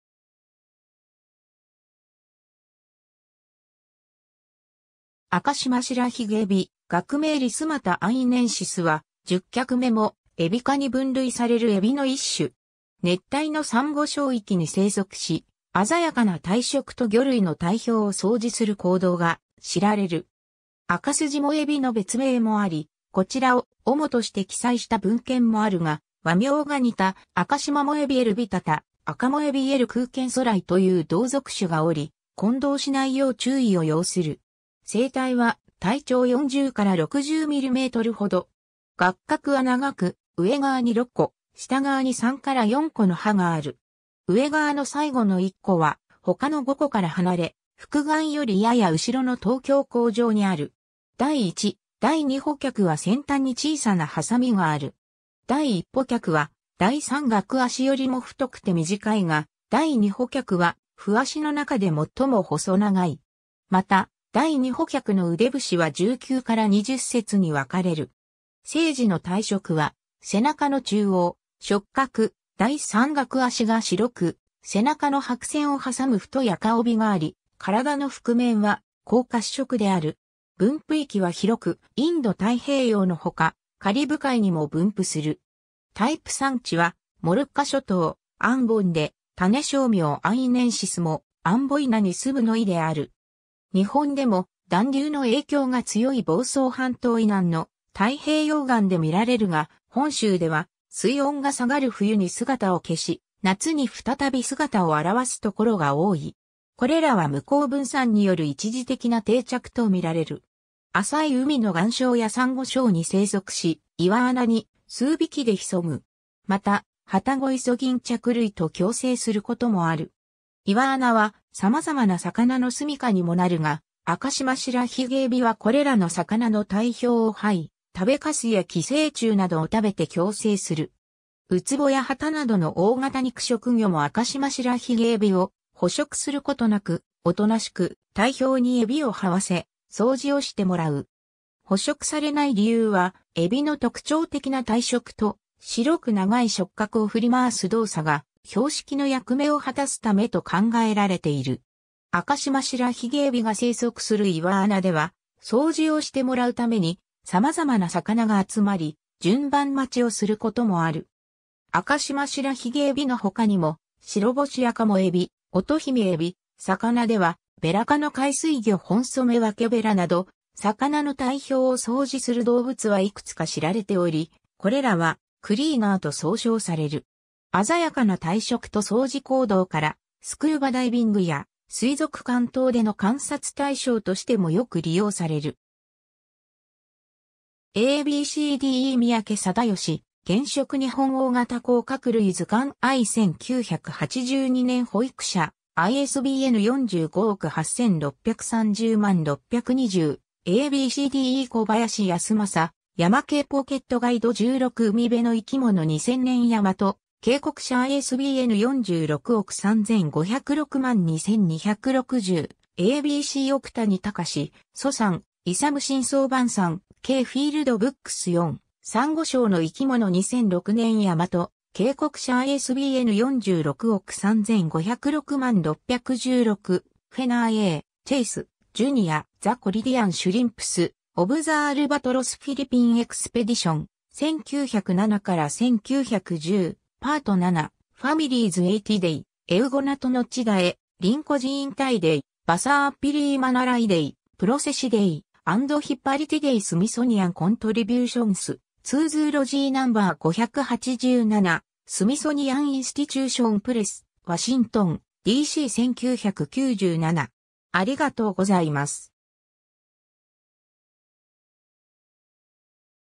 アカシマシラヒゲエビ
アカシマシラヒゲエビ（赤縞白髭蝦）、学名 Lysmata amboinensis は、十脚目モエビ科に分類されるエビの一種。
熱帯のサンゴ礁域に生息し、鮮やかな体色と魚類の体表を掃除する行動が知られる。
アカスジモエビ（赤条藻蝦）の別名もあり、こちらを主として記載した文献もあるが、和名が似たアカシマモエビ L. vittata、アカモエビ L. kuekenthali という同属種がおり、混同しないよう注意を要する。
 成体は体長40-60mmほど。
額角は長く、上側に6個、下側に3-4個の歯がある。
上側の最後の1個は他の5個から離れ、複眼よりやや後ろの頭胸甲上にある。
第1・第2歩脚は先端に小さなはさみがある。
第1歩脚は第3顎脚よりも太くて短いが、第2歩脚は歩脚の中で最も細長い。
また第2歩脚の腕節は19-20節に分かれる。
生時の体色は背中の中央、触角、第3顎脚が白く、背中の白線を挟む太い赤帯があり、体の腹面は黄褐色である。
 分布域は広く、インド太平洋の他、カリブ海にも分布する。
タイプ産地はモルッカ諸島・アンボン（アンボイナ）で、種小名"amboinensis"も「アンボイナに棲む」の意である。
日本でも暖流の影響が強い房総半島以南の太平洋岸で見られるが、本州では水温が下がる冬に姿を消し、夏に再び姿を現す所が多い。
これらは無効分散（死滅回遊）による一時的な定着とみられる。
 浅い海の岩礁やサンゴ礁に生息し、岩穴に数匹で潜む。
また、ハタゴイソギンチャク類と共生することもある。
 岩穴は様々な魚の住処にもなるが、アカシマシラヒゲエビはこれらの魚の体表を這い、食べかすや寄生虫などを食べて共生する。
ウツボやハタなどの大型肉食魚もアカシマシラヒゲエビを捕食することなく、おとなしく体表にエビを這わせ、掃除をしてもらう。
捕食されない理由は、エビの特徴的な体色と、白く長い触角を振り回す動作が標識の役目を果たすためと考えられている。
アカシマシラヒゲエビが生息する岩穴では、掃除をしてもらうために様々な魚が集まり、「順番待ち」をすることもある。
アカシマシラヒゲエビの他にもシロボシアカモエビ、オトヒメエビ、魚ではベラ科の海水魚ホンソメワケベラなど、魚の体表を掃除する動物はいくつか知られており、これらはクリーナー（Cleaner）と総称される。
 鮮やかな体色と掃除行動から、スクーバダイビングや水族館等での観察対象としてもよく利用される。
 ^ a b c d e 三宅貞祥『原色日本大型甲殻類図鑑 I』1982年 保育社 ISBN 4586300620 ^ a b c d e 小林安雅『ヤマケイポケットガイド16 海辺の生き物』2000年 山と溪谷社 ISBN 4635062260 ^ a b c 奥谷喬司・楚山勇 新装版山溪フィールドブックス4『サンゴ礁の生きもの』（解説:武田正倫）2006年 山と溪谷社 ISBN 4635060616 ^ Fenner A. Chace, Jr."The Caridean Shrimps (Crustacea:Decapoda) of the Albatross Philippine Expedition, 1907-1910, Part 7: Families Atyidae, Eugonatonotidae, Rhynchocinetidae, Bathypalaemonellidae, Processidae, and Hippolytidae" Smithsonian Contributions to Zoology No. 587,Smithsonian Institution Press, Washington, D.C. 1997